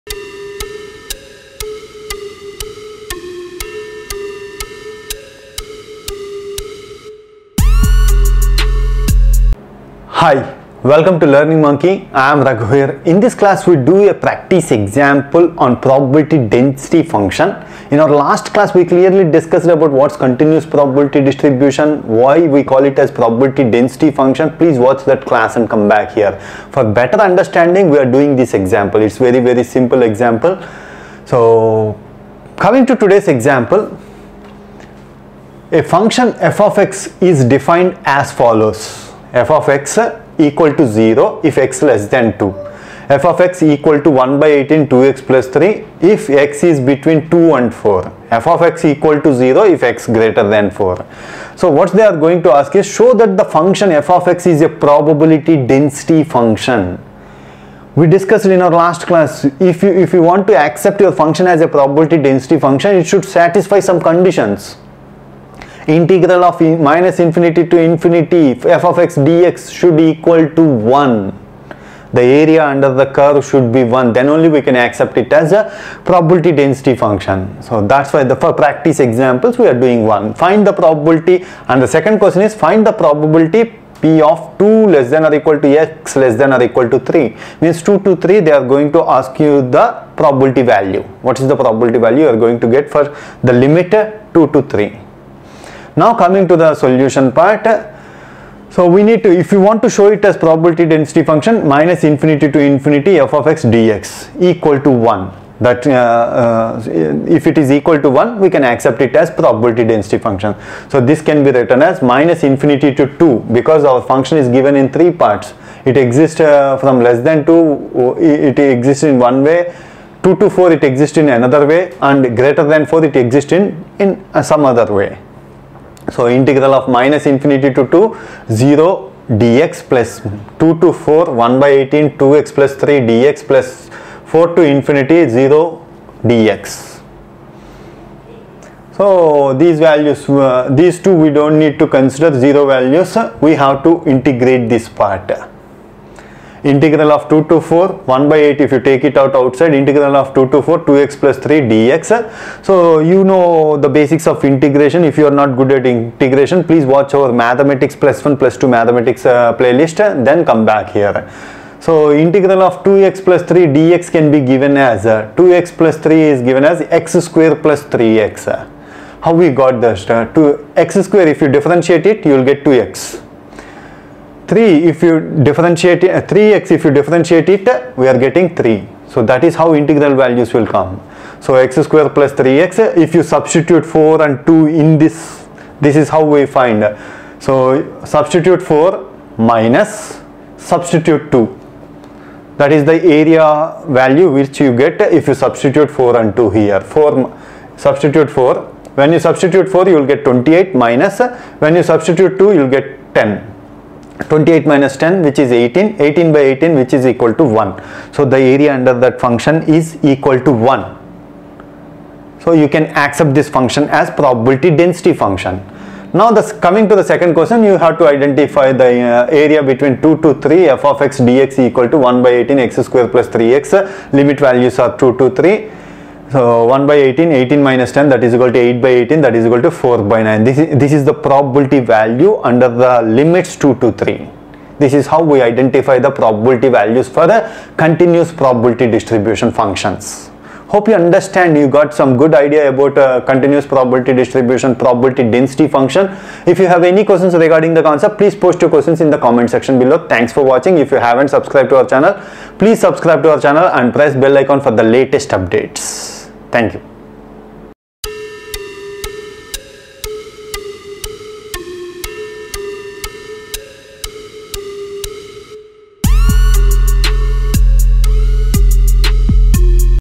Probability Density Function or not Example | Lesson 62 | Probability & Statistics | Learning Monkey
Probability Density Function or not Example
In this class, We discuss Probability Density Function or not Example.
The reader should have prior knowledge of continuous probability distribution. Click Here.
Example 1:
A function f(x) is defined as follows.
f(x) = 0 when x lt2
= 1/18(2x+ 3) when 2 lt= x lt= 4
= 0 when x gt 4
1) Show that f(x) is a probability density function?
2) Find the probability P(2 lt= x lt= 3)?
Solution:
The below equation shows the area under the function f(x) = 1. To accept f(x) as a probability density function.
∫_(-∞)^∞▒f(x)dx
= ∫_(-∞)^2▒0dx +  ∫_2^4▒〖1/18(2x+3)dx〗 + ∫_4^∞▒〖0 dx〗
= 1/18 ∫_2^4▒〖2x+3〗
= 1/18[x2 + 3x]42
= 1/18[28-10]
= 1

∫_2^3▒f(x)dx = 1/18[x2 + 3x]32
= 1/18[18 – 10]
= 8/18
= 4/9